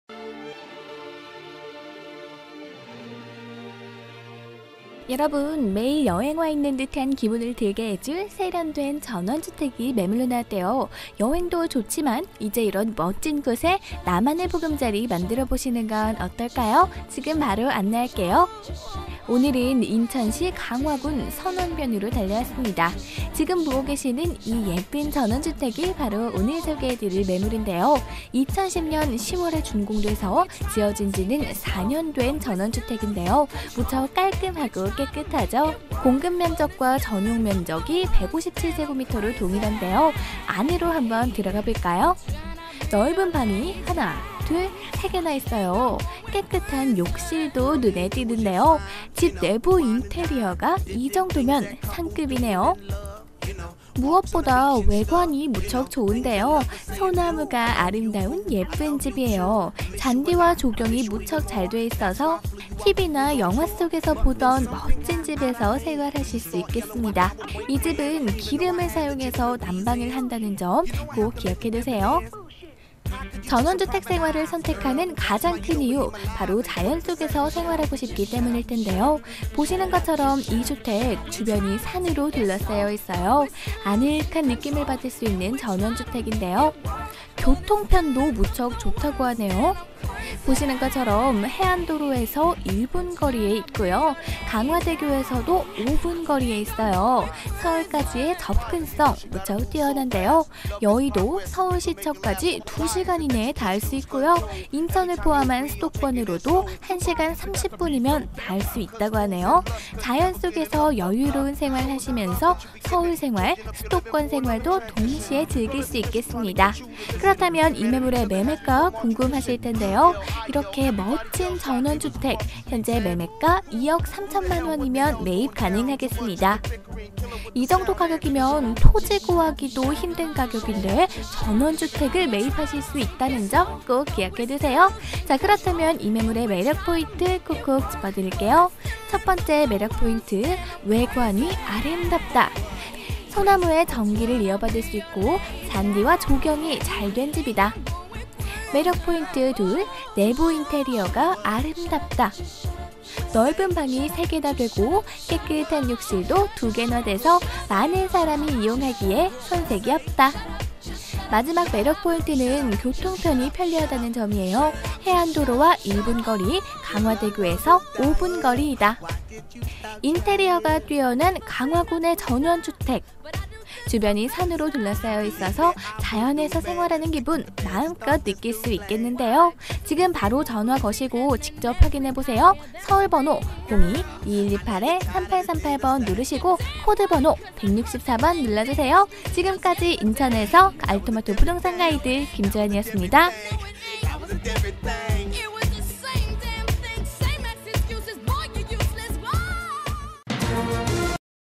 [거래완료 주택]선원면 전원주택
강화대교와 가까운 아름다운집

- 강화대교에서 5분거리에 있는 집
- 주변 산으로 둘러 쌓인 아늑한 전원주택
- 해안도로에서 1분거리에 있음.
- 소나무가 아름다운 예쁜 집
- 잔디와 조경이 잘 된 교통좋은 집
- 집 내부 인테리어가 상급!!